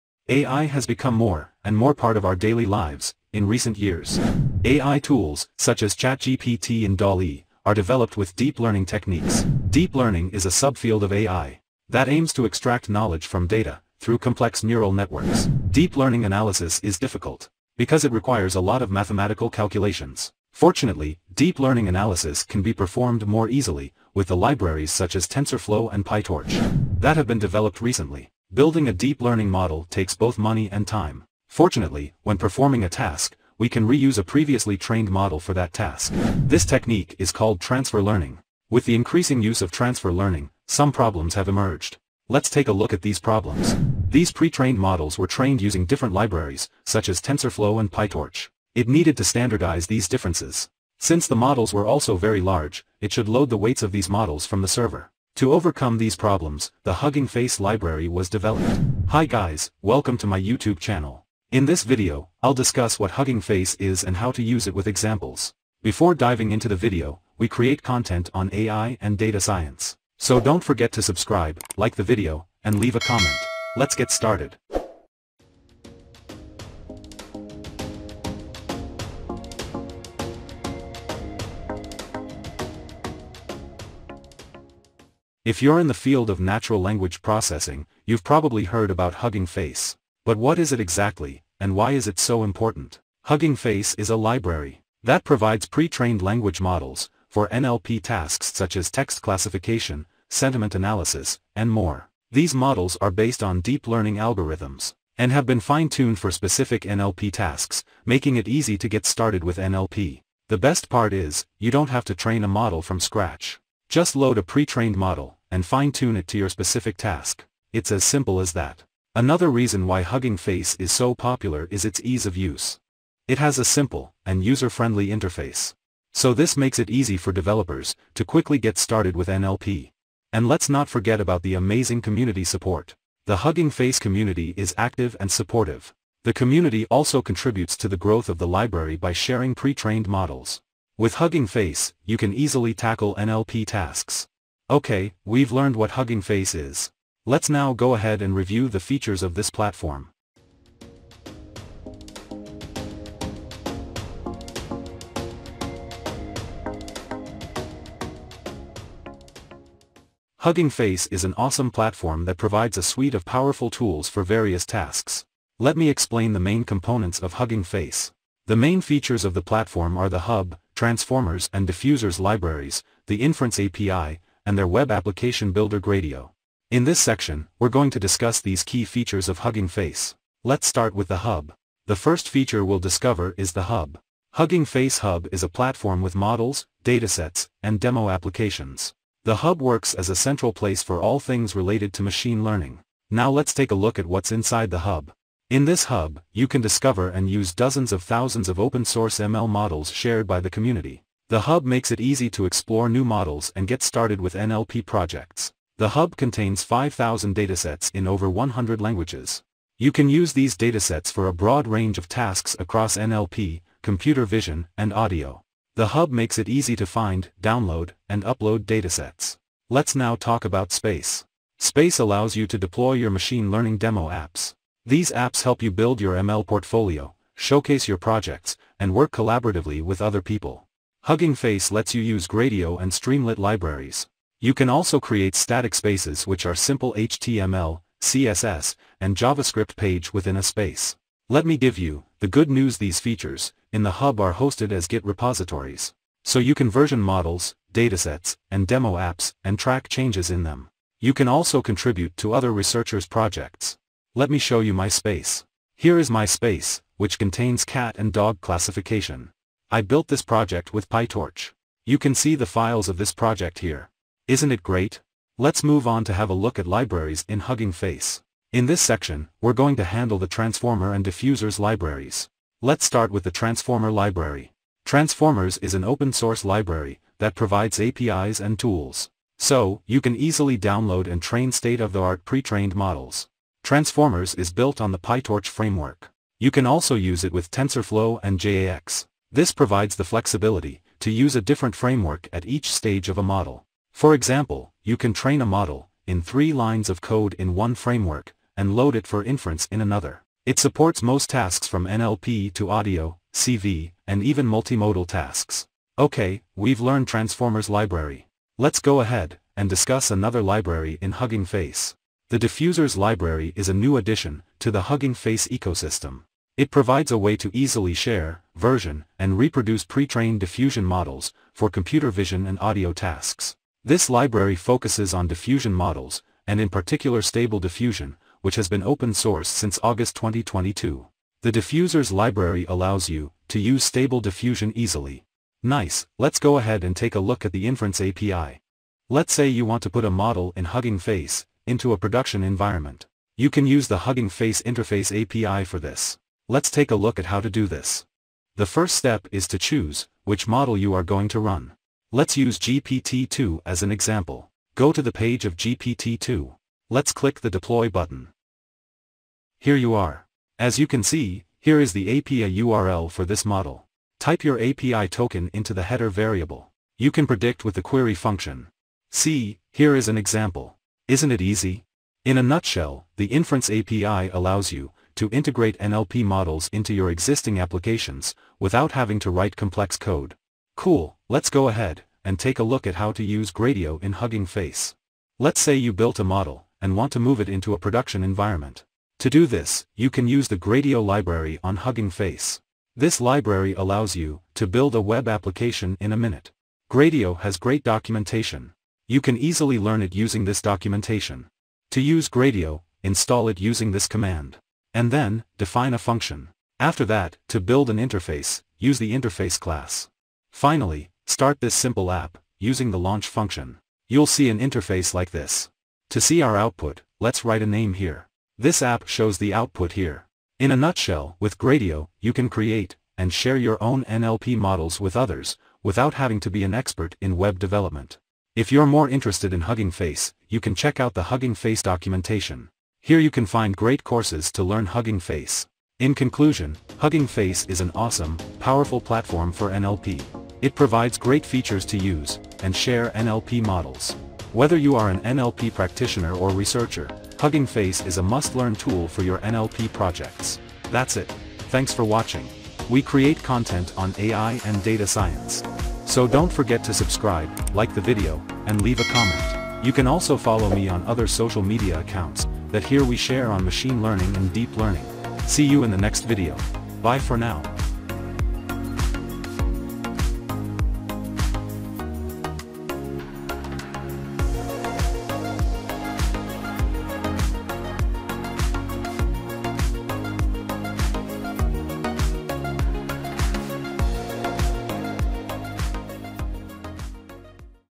Getting Started with HUGGING FACE 🔥 AI Tutorial
What is Hugging Face & How to Use | AI Framework | Data Science & NLP Tutorial | Machine Learning & Deep Learning with Python

Hi Guys, Welcome to Tirenadaz Academy 😀

This video walks you through getting started with Hugging Face.

00:11 What is deep learning?
00:37 What is transfer learning?
00:46 Why Hugging Face?
01:21 What is Hugging Face?
01:51 Advantages of Hugging Face
02:37 The Hugging Face ecosystem
03:03 The Hub
04:30 The Transformers and Diffusion libraries
05:44 The Interface API
06:25 Gradio

Happy learning 🚀